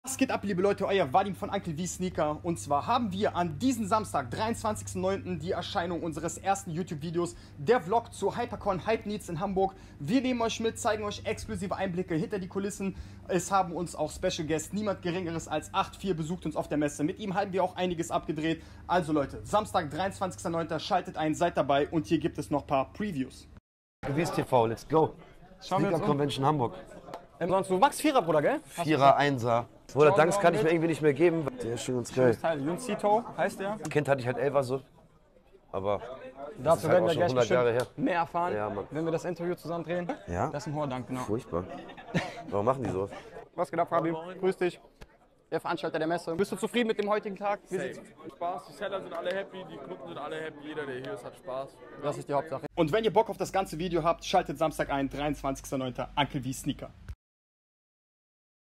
Teaser zu meinem ersten YouTube Video am 23.09.23! Vlog zur Hypercon mit @AchtVier & ⁠@HYPENEEDZ
Ich zeige euch exklusive Einblicke hinter die Kulissen als Seller von der Sneakermesse in Hamburg im Alsterhaus und haben darüber hinaus jede Menge Sneakercontent und Sneakertalk für euch. Auch @AchtVier hat uns einen Besuch abgestattet und mit uns was abgedreht, also auch hier an die HipHop Fans, seid gespannt was Achtvier so über Sneaker zu erzählen hat und was seine Lieblingssneaker sind oder welche er zum Beispiel garnicht feiert!

Seid gespannt und schaltet ein! Ich freue mich über jeden Support 🙌❤️